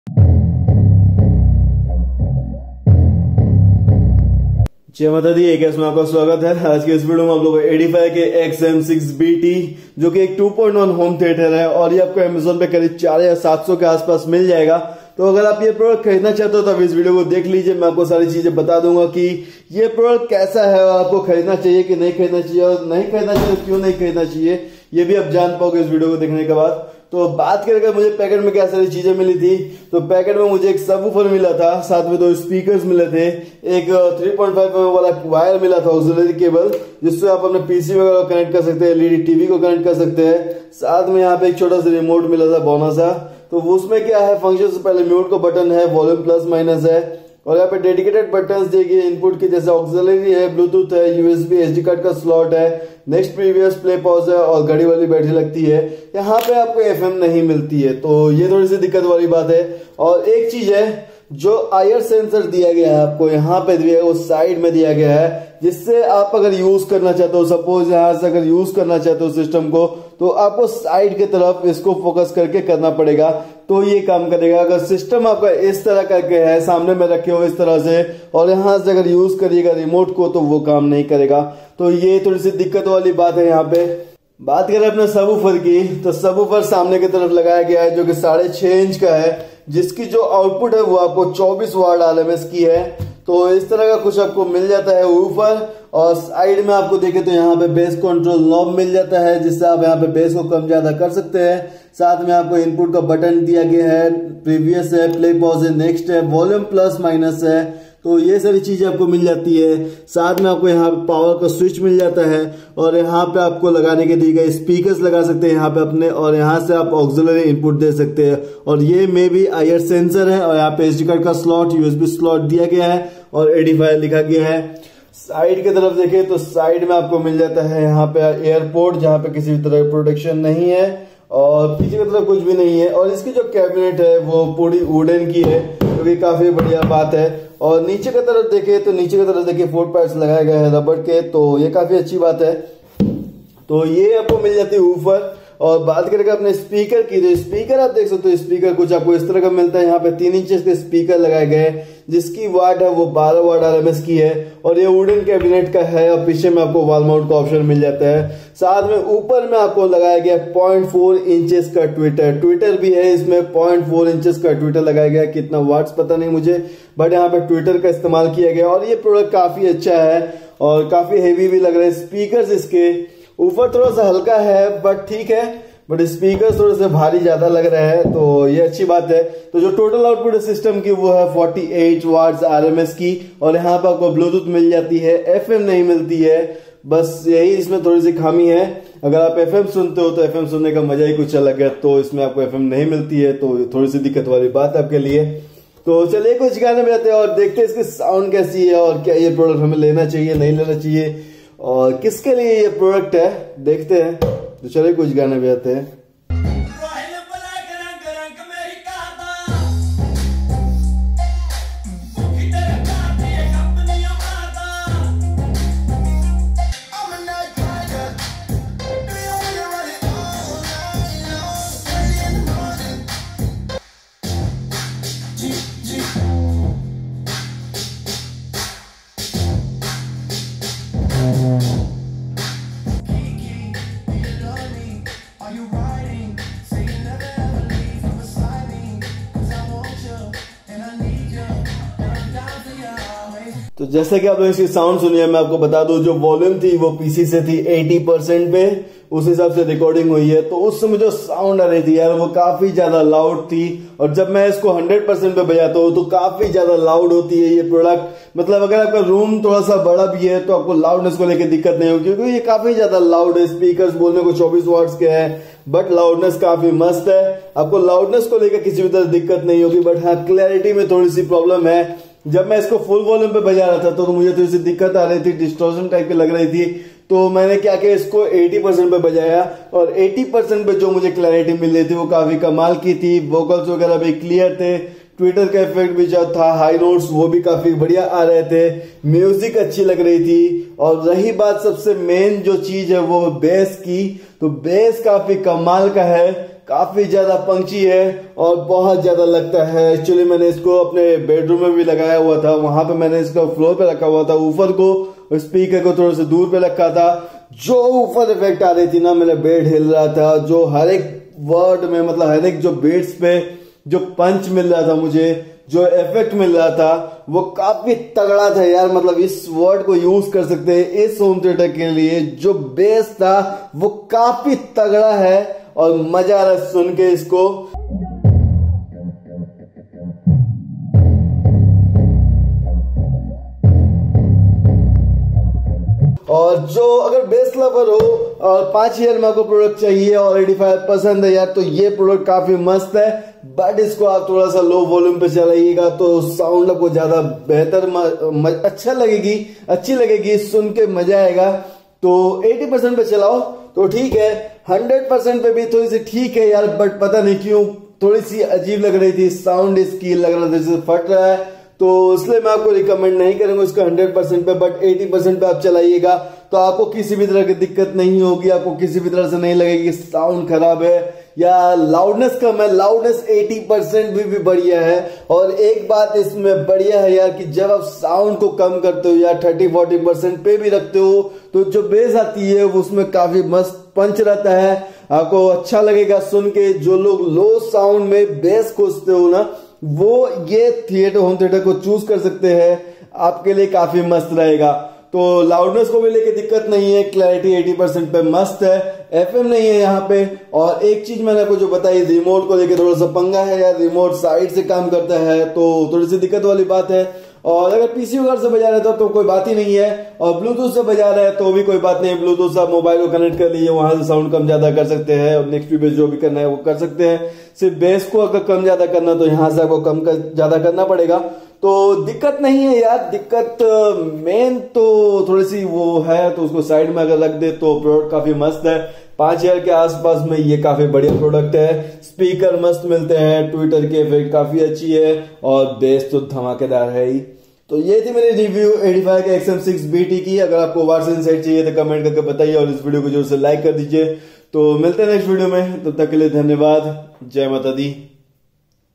Edifier XM6BT 2.1 Home Theatre System Review by AKS
UnBoxing, Sound Test & Review of Edifier SPK-EF-XM6BT 2.1 Bluetooth Multimedia Speaker System  by AKS

Edifier SPK-EF-XM6BT 2.1

With Bluetooth function, USB Flash Disk /SD card play back, AUX Input
6 1/2 inch bass and 3 inch mid-range driver , 10mm Tweeter
Integrated side control panel makes system adjustments easy and convenient
With IR remote control to select source directly
Six buttons for audio source selection, previous/next track, play/pause and volume up/down